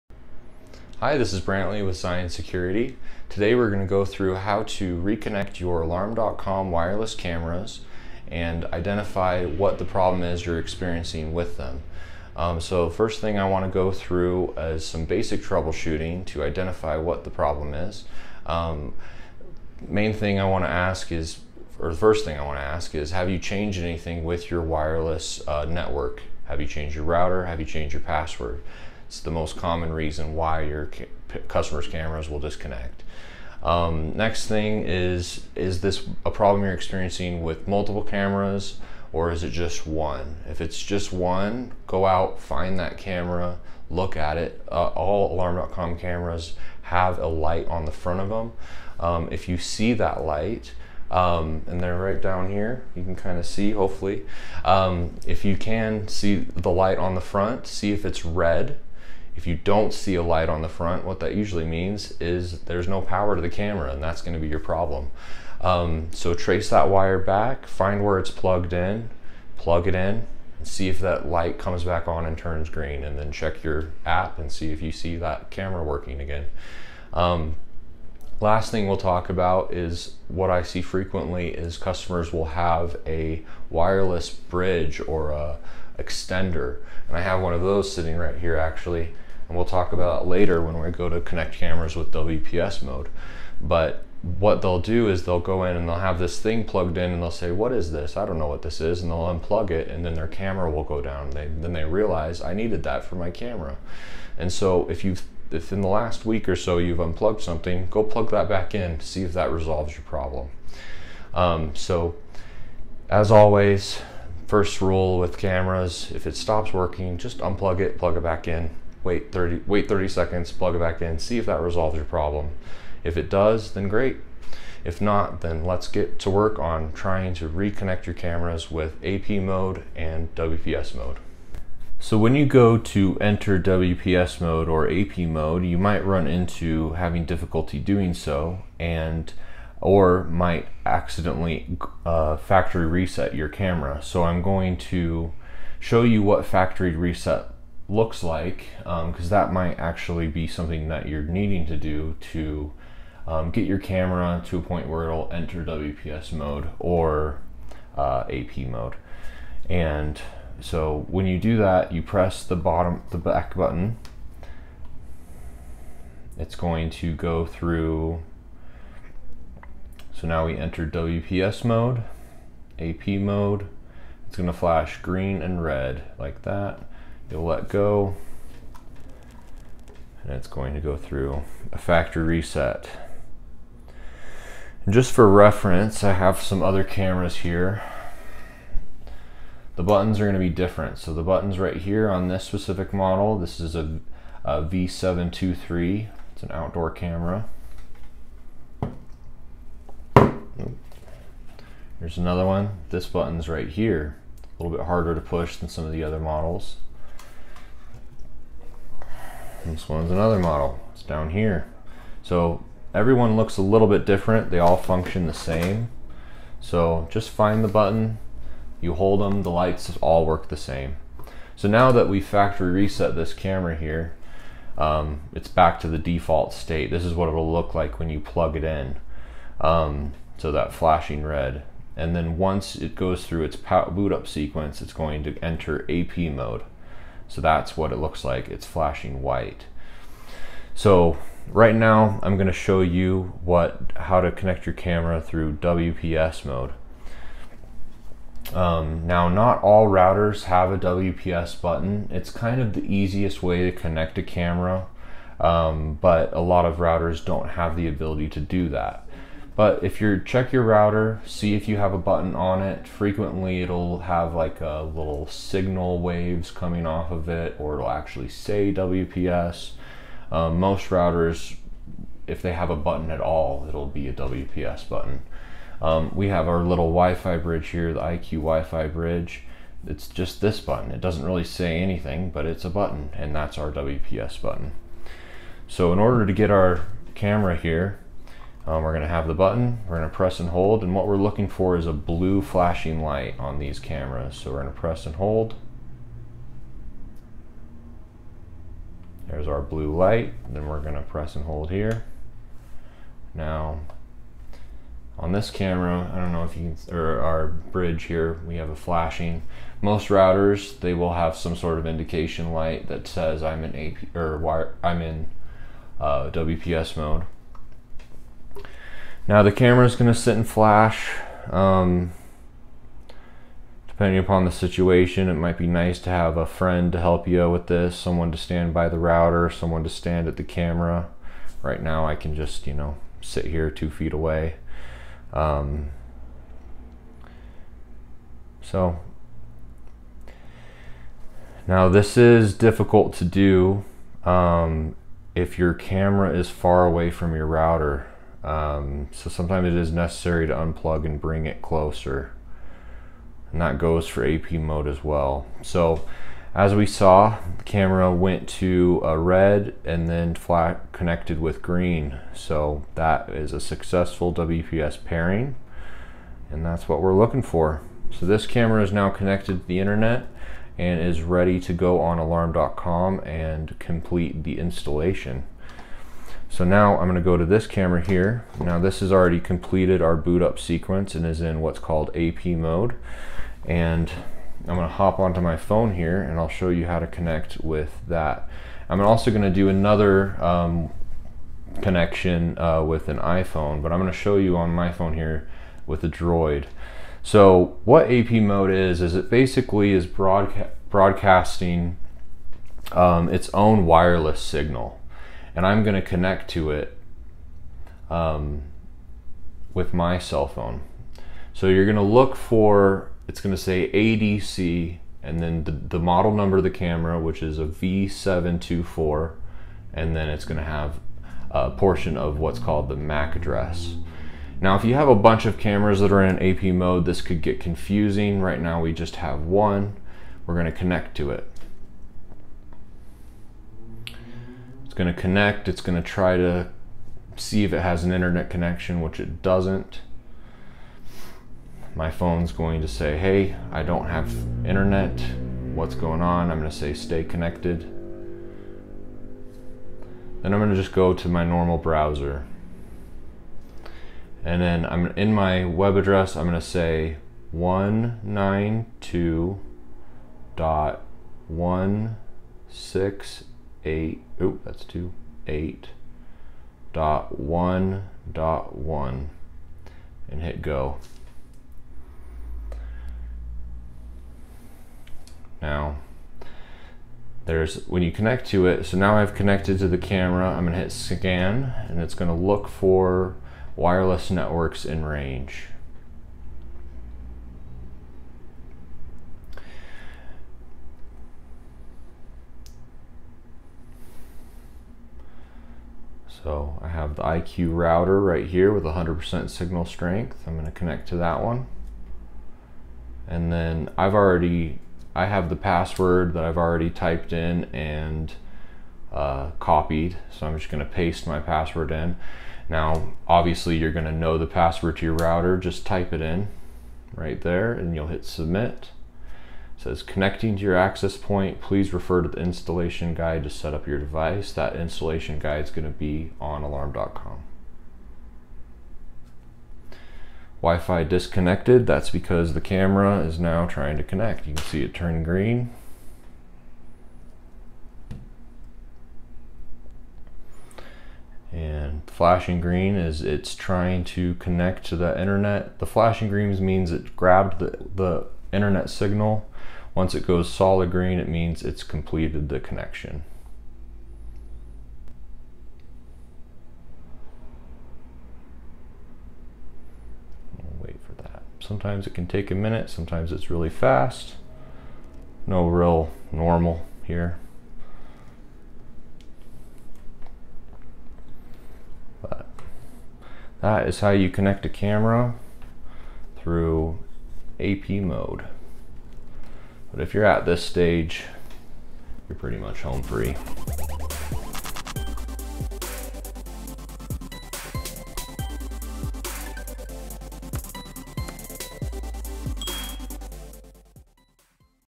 How to Reconnect Your Alarm.com Cameras to WiFi if They Stop Working
If you have Alarm.com cameras and they stop working this video will run you through how to get them connected again to your WiFi.